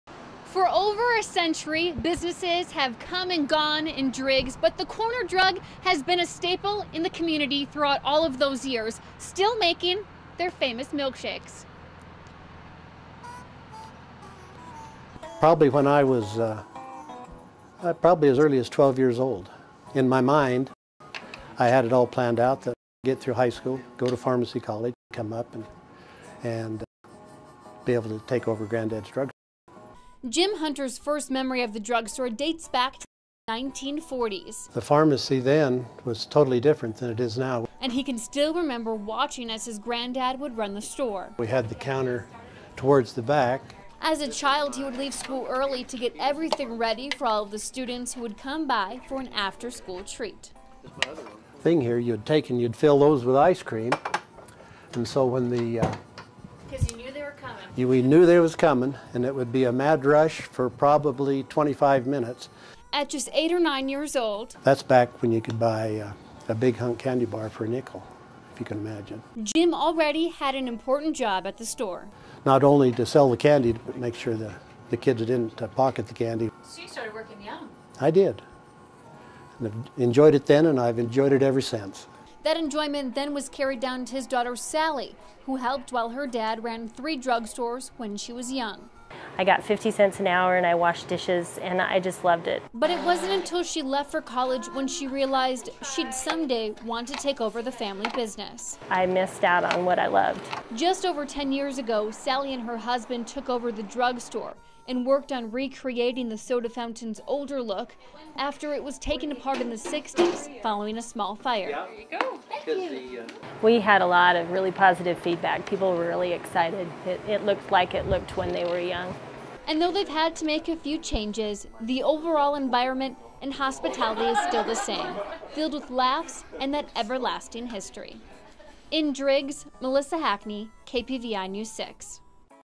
Corner Drug in Driggs
For this backyard gem I traveled to the over century old Corner Drug in Driggs, ID.